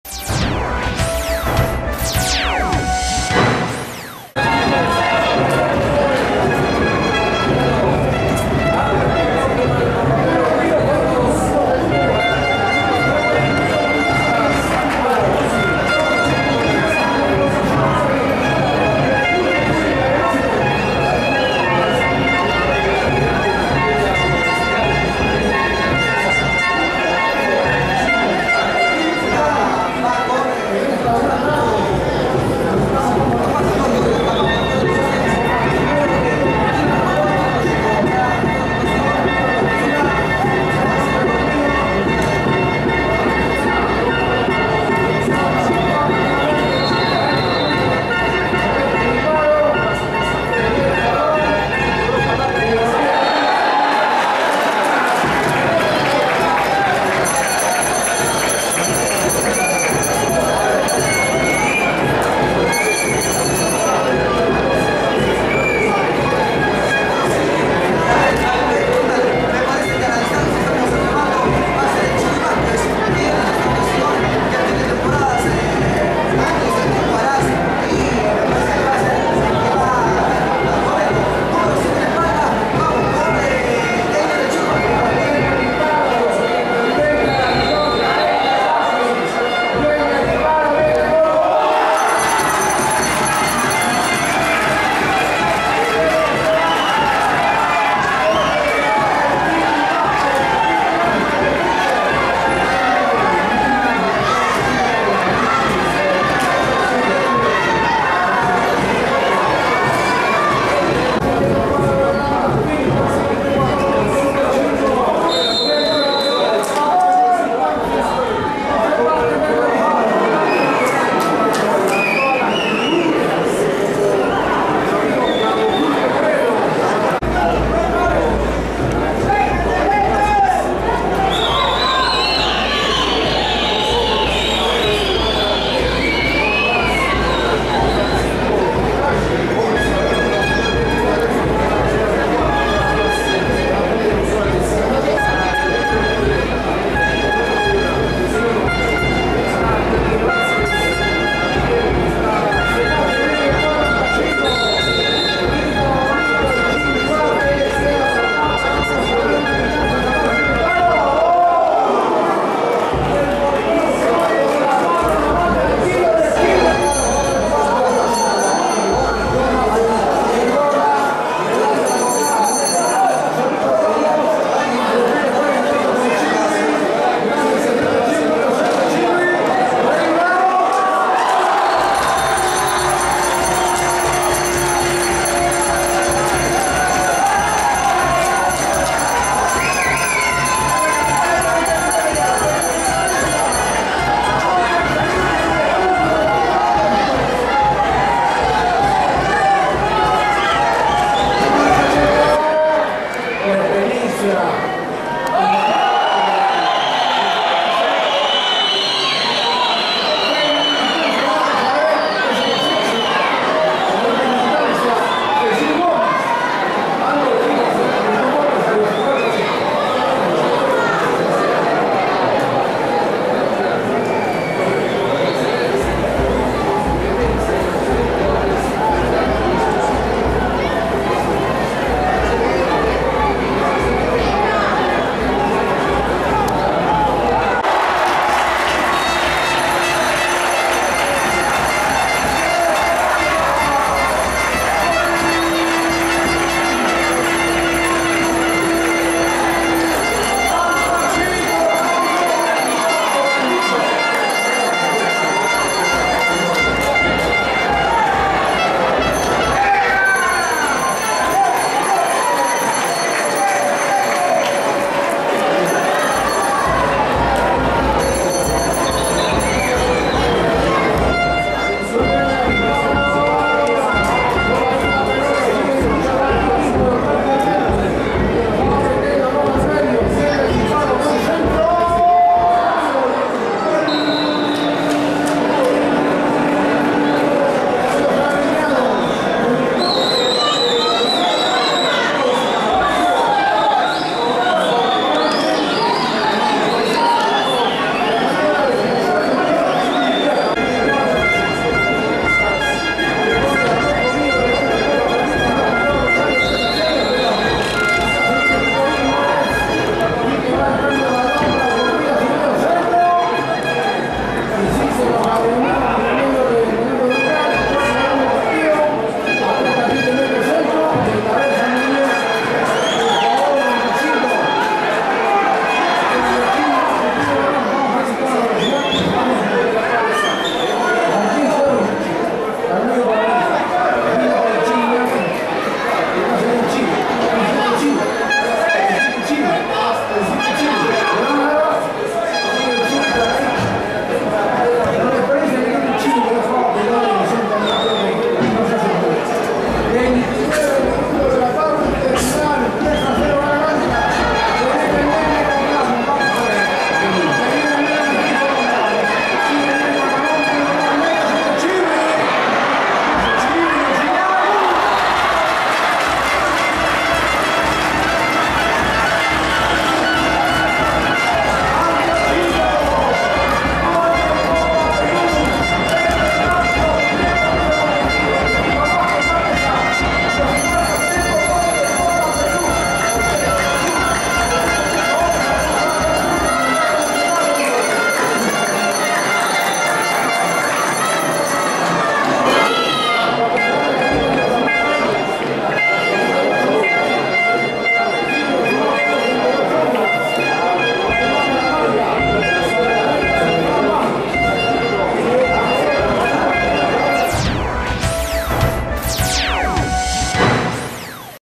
SPORT ANCASH FC 4 VS AGREMIADOS 1 EN HUARAZ
EL ANCASH GANO 4 A 1 EN SU PRIMER PARTIDO DE PREPARACIÓN CON MIRAS AL INICIO DE LA SEGUNDA DIVISIÓN POR 4 A 1